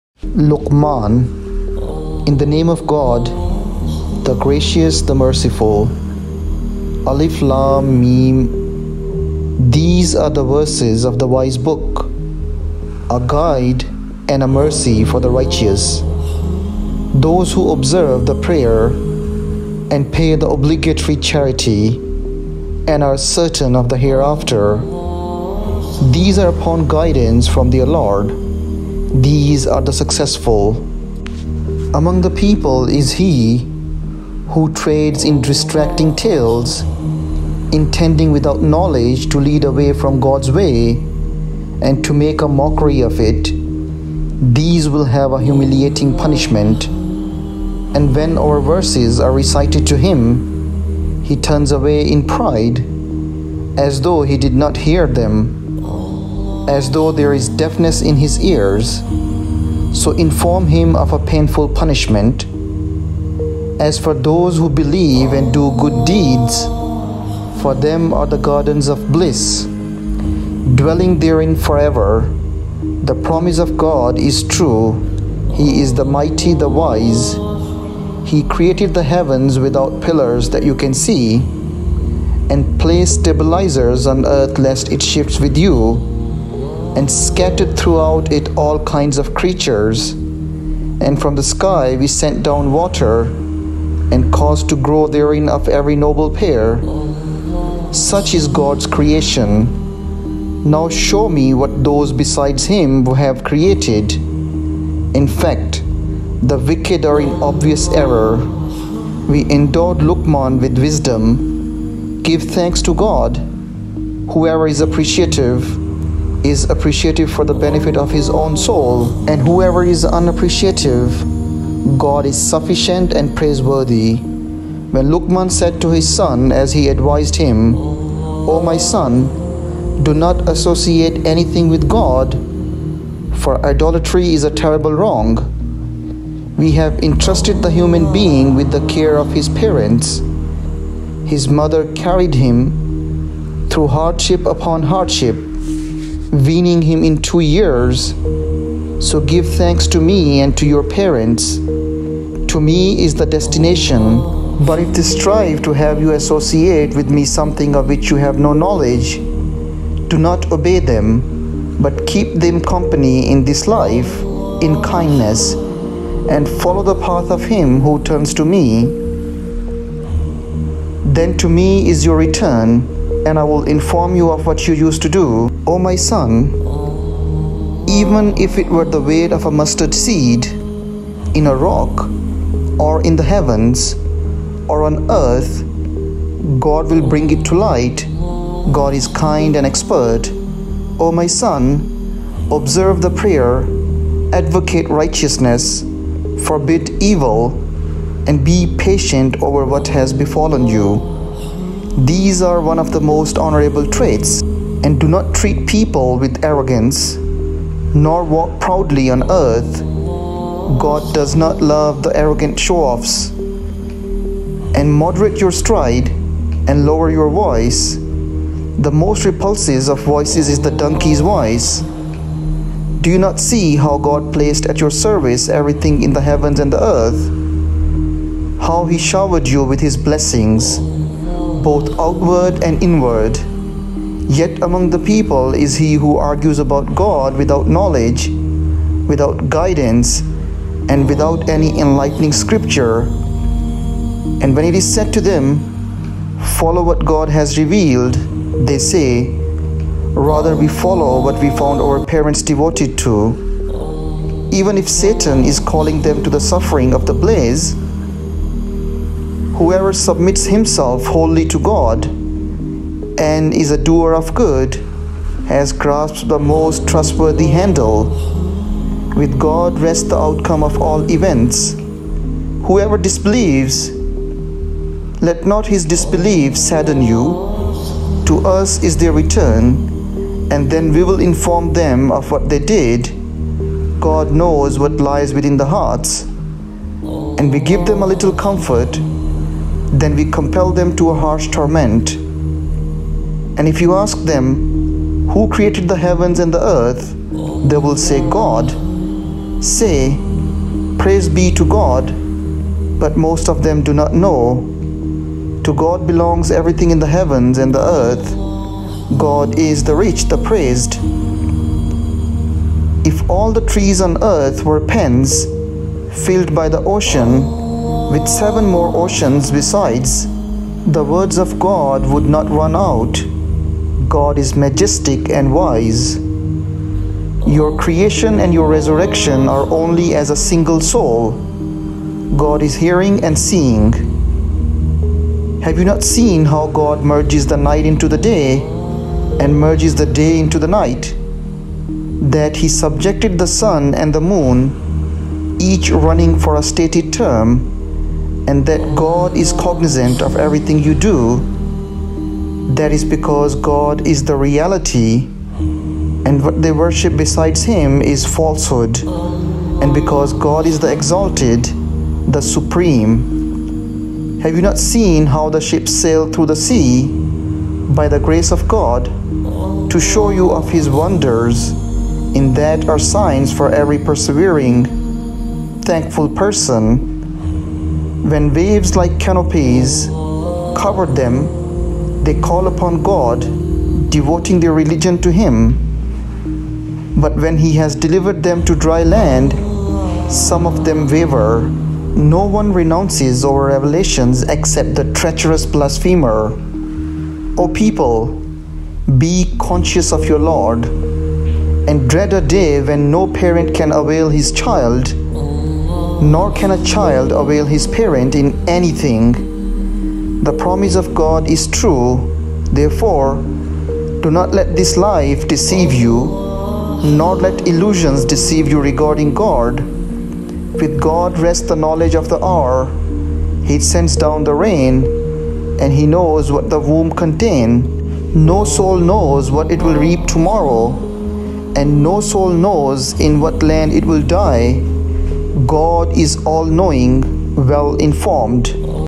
31 Surah Luqman , Ayat 1-34 (English Quran)/ Chapter 31 : Luqman , verse 1-34
Chapter 31 : Luqman 1-34

In the name of God, the Gracious, the Merciful.

1. Alif, Lam, Meem.

2. These are the Verses of the Wise Book.

3. A guide and a mercy for the righteous.

4. Those who observe the prayer, and pay the obligatory charity, and are certain of the Hereafter.

5. These are upon guidance from their Lord. These are the successful.

6. Among the people is he who trades in distracting tales; intending, without knowledge, to lead away from God’s way, and to make a mockery of it. These will have a humiliating punishment.

7. And when Our Verses are recited to him, he turns away in pride, as though he did not hear them, as though there is deafness in his ears. So inform him of a painful punishment.

8. As for those who believe and do good deeds—for them are the Gardens of Bliss.

9. Dwelling therein forever. The promise of God is true. He is the Mighty, the Wise.

10. He created the heavens without pillars that you can see, and placed stabilizers on earth lest it shifts with you, and scattered throughout it all kinds of creatures. And from the sky We sent down water, and caused to grow therein of every noble pair.

11. Such is God’s creation. Now show me what those besides Him have created. In fact, the wicked are in obvious error.

12. We endowed Luqman with wisdom: “Give thanks to God.” Whoever is appreciative—is appreciative for the benefit of his own soul. And whoever is unappreciative—God is Sufficient and Praiseworthy.

13. When Luqman said to his son, as he advised him, “O my son, do not associate anything with God, for idolatry is a terrible wrong.”

14. We have entrusted the human being with the care of his parents. His mother carried him through hardship upon hardship, weaning him in two years. So give thanks to Me, and to your parents. To Me is the destination.

15. But if they strive to have you associate with Me something of which you have no knowledge, do not obey them. But keep them company in this life, in kindness, and follow the path of him who turns to Me. Then to Me is your return; and I will inform you of what you used to do.

16. “O my son, even if it were the weight of a mustard-seed, in a rock, or in the heavens, or on earth, God will bring it to light. God is Kind and Expert.

17. O my son, observe the prayer, advocate righteousness, forbid evil, and be patient over what has befallen you. These are of the most honorable traits.

18. And do not treat people with arrogance, nor walk proudly on earth. God does not love the arrogant showoffs.

19. And moderate your stride, and lower your voice. The most repulsive of voices is the donkey’s voice.”

20. Do you not see how God placed at your service everything in the heavens and the earth? How He showered you with His blessings, both outward and inward? Yet among the people is he who argues about God without knowledge, without guidance, and without an enlightening Scripture.

21. And when it is said to them, “Follow what God has revealed,” they say, “Rather, we follow what we found our parents devoted to.” Even if Satan is calling them to the suffering of the Blaze?

22. Whoever submits himself wholly to God, and is a doer of good, has grasped the most trustworthy handle. With God rests the outcome of all events.

23. Whoever disbelieves—let not his disbelief sadden you. To Us is their return. Then We will inform them of what they did. God knows what lies within the hearts.

24. We give them a little comfort; then We compel them to a harsh torment.

25. And if you ask them, “Who created the heavens and the earth?” They will say, “God.” Say, “Praise be to God.” But most of them do not know.

26. To God belongs everything in the heavens and the earth. God is the Rich, the Praised.

27. If all the trees on earth were pens, filled by the ocean, with seven more oceans besides, the Words of God would not run out. God is Majestic and Wise.

28. Your creation and your resurrection are only as a single soul. God is Hearing and Seeing.

29. Have you not seen how God merges the night into the day, and merges the day into the night? That He subjected the sun and the moon, each running for a stated term? And that God is Cognizant of everything you do?

30. That is because God is the Reality, and what they worship besides Him is falsehood, and because God is the Exalted, the Supreme.

31. Have you not seen how the ships sail through the sea, by the grace of God, to show you of His wonders? In that are signs for every persevering, thankful person.

Quran Translation: Talal Itani, ClearQuran.
Photo Credit: Wallpaper Cave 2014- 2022.
Background Sound Credit: Relaxing Music for Spiritual Healing & Meditation